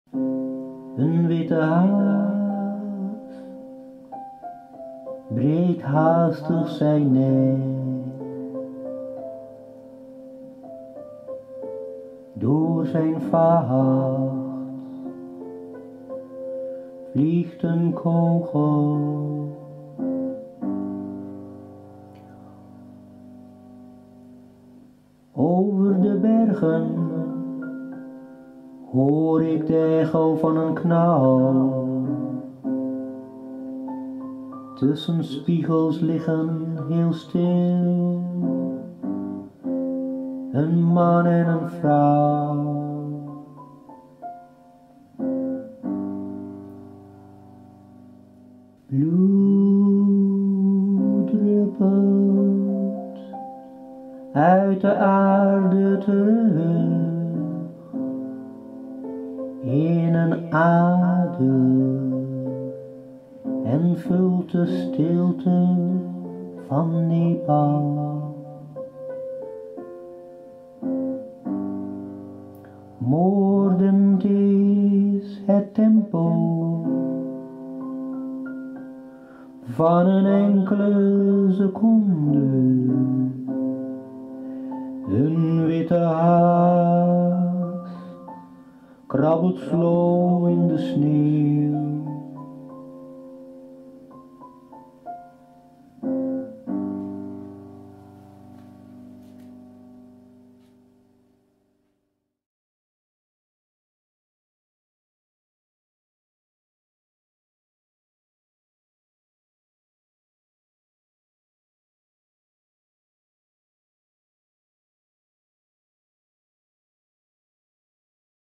Dreamsjamaan - Slapen in de stille sneeuw van Nepal
'Slapen in de stille sneeuw van Nepal' is een gedicht uit het boekje 'De Schittering'.
Copyright
Auteur                : Dreojs Amennob
Uitgeverij           : De Schitterende Hagedis
Muziek /piano  : Dreamsjamaan

Slapen in de stille sneeuw van Nepal

Een witte haas
breekt haastig zijn nek,
door zijn vacht
vliegt een kogel

Over de bergen hoor ik
de echo van een knal,
tussen spiegels liggen, heel stil
een man en een vrouw

Bloed druppelt
uit de aarde terug
in een ader en vult
de stilte van Nepal

Moordend is het tempo
van een enkele seconde;
een witte haas
krabbelt sloom in  de sneeuw